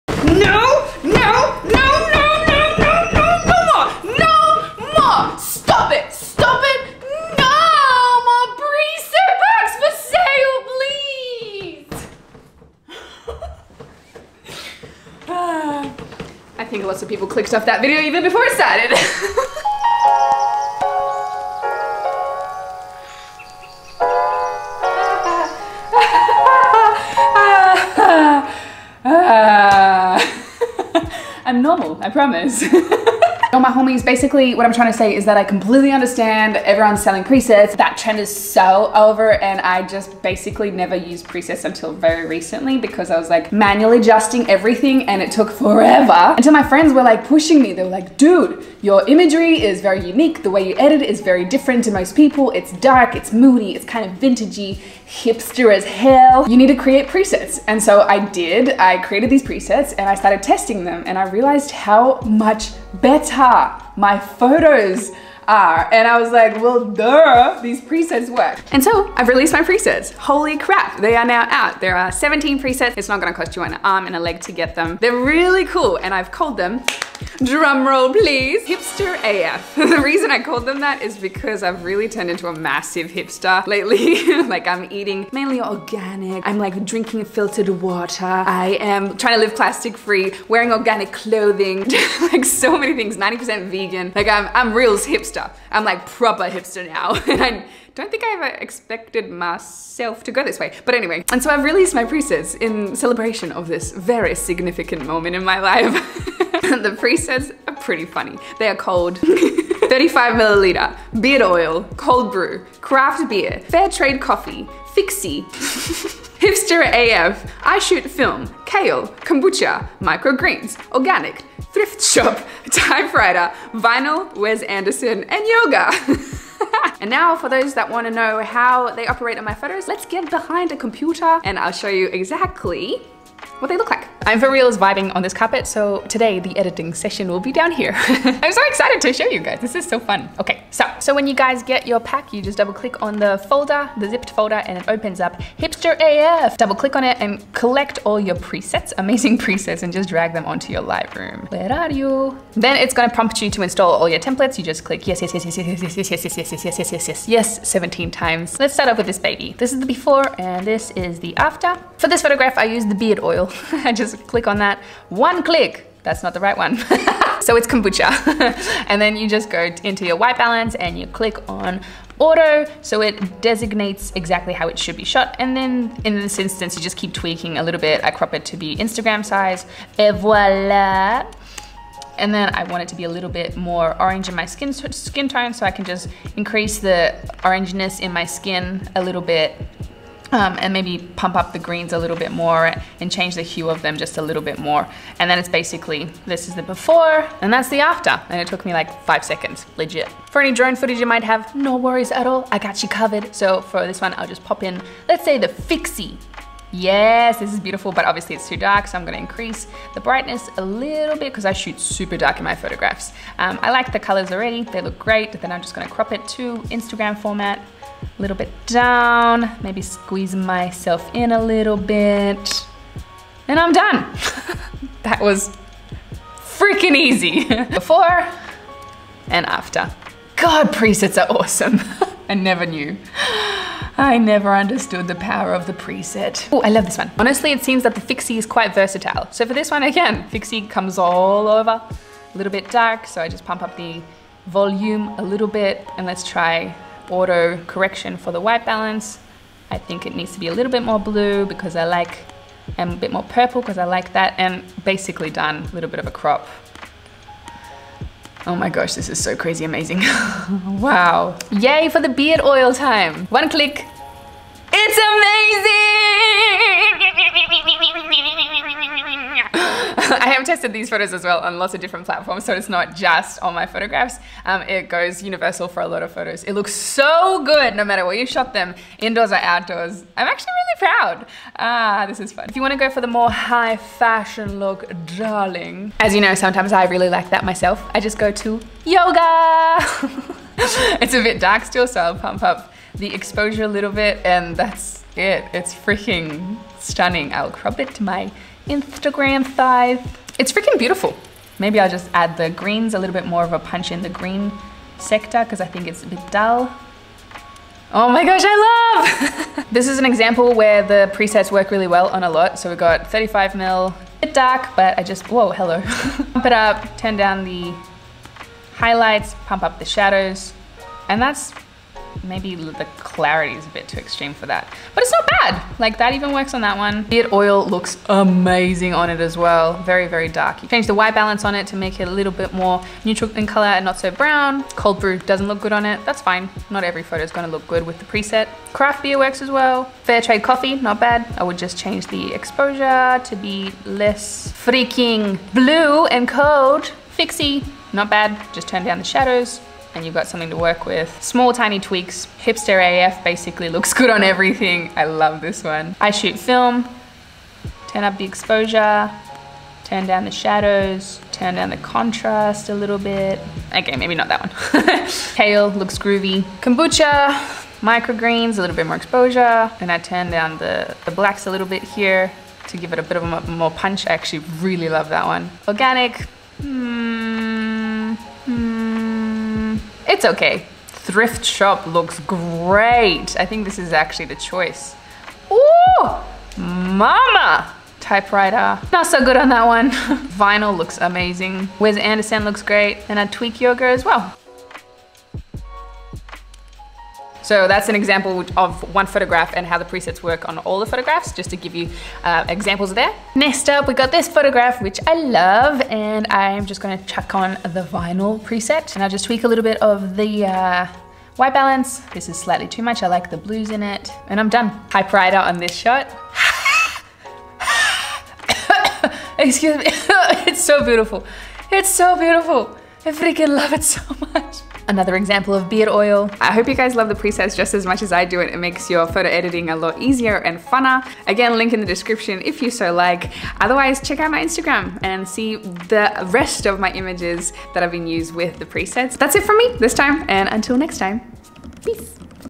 Hipster Photo Editing: What Are Presets And Should You Use Them For Your Photography?
If you’ve ever looked at my photos and wondered how I edit them, wanted to copy my editing style, or just needed some sweet Lightroom presets to give all your photos that extra moody vibe, I’m very happy to present to you all 17 super sexy presets that I use on almost all of the photos you see on my own Instagram.

If you know my photo style, you know I like to go for darker tones than what’s the standard vibe out there, and that’s generally what you’ll find here – although around a third of the presets are lighter and brighter when your mood is a little more upbeat, or need something to suit a brighter environment.

I decided to call them Hipster AF. Ha! To celebrate my new major hipsterification where I eat mainly organic, try to live plastic free as much as possible, I filter all my water, I am 90% vegan and I am starting to wear only second hand or organic clothing...this momentous occasion called for a preset pack!

I’d love to see what you come up with when using them, so feel free to tag me on Instagram @sorelleamore and use the hashtag SorelleAmorePresets so I can have a look at your sexy new hipster photos! Enjoy my lovers!